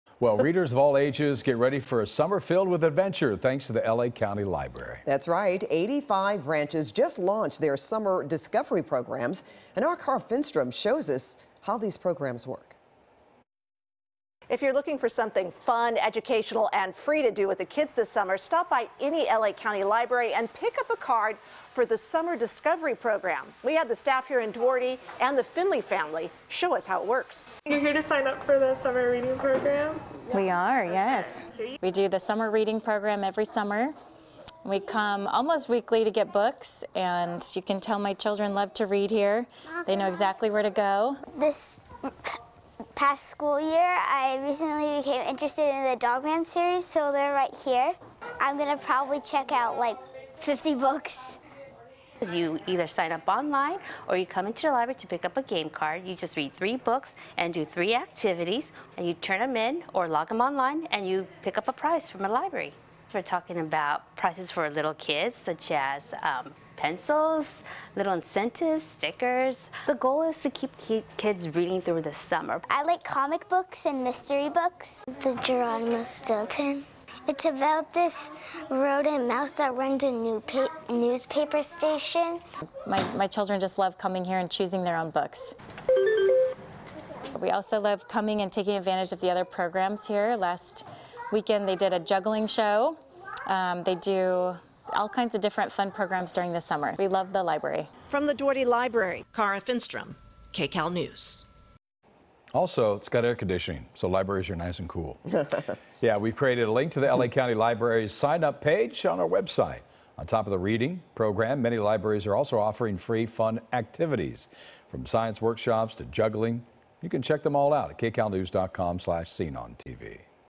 LA County Libraries launch summer program filled with adventure
Readers of all ages, get ready for a summer filled with adventure. Kara Finnstrom reports.